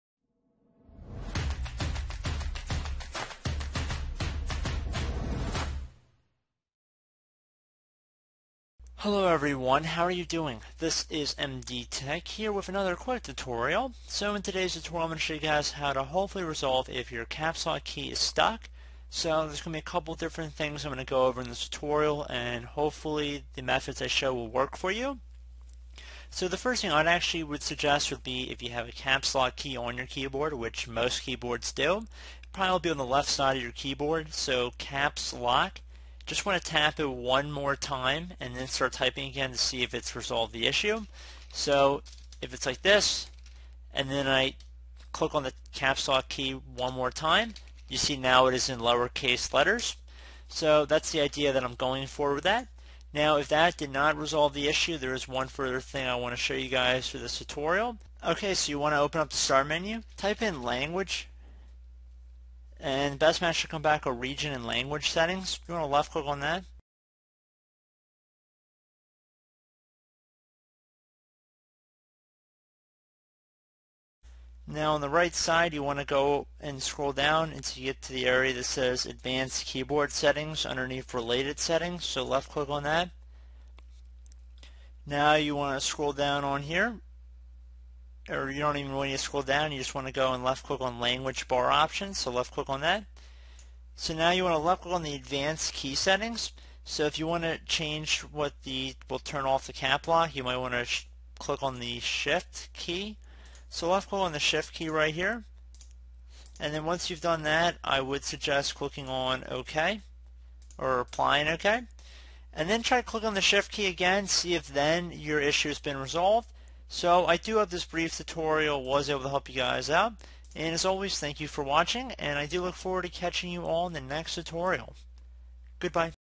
Verrouillage des majuscules bloqué dans Windows 10
Coincé avec CAPS LOCK Fix - Comment réécrire en minuscules [Tutoriel]

Caps Lock, qui vous oblige à taper toutes les lettres majuscules, sur un ordinateur Windows ou Mac. Désactiver le verrouillage des majuscules sur un ordinateur fonctionnel est aussi simple que d'appuyer à nouveau sur la touche « Verrouillage des majuscules », mais si votre touche de verrouillage des majuscules est bloquée, vous devrez réparer la clé.

Ce didacticiel s'appliquera aux ordinateurs, ordinateurs portables, ordinateurs de bureau et tablettes exécutant les systèmes d'exploitation Windows 11, Windows 10, Windows 8/8.1, Windows 7. Fonctionne pour tous les principaux fabricants d'ordinateurs (Dell, HP, Acer, Asus, Toshiba, Lenovo, Samsung).